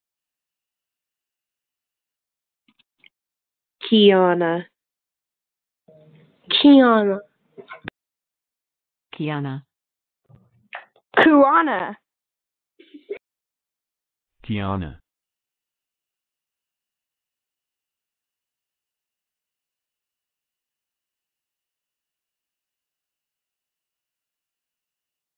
How to Pronounce Keyana
This video shows you how to pronounce Keyana. Record your own pronunciation, view the origin, meaning, and history of the name Keyana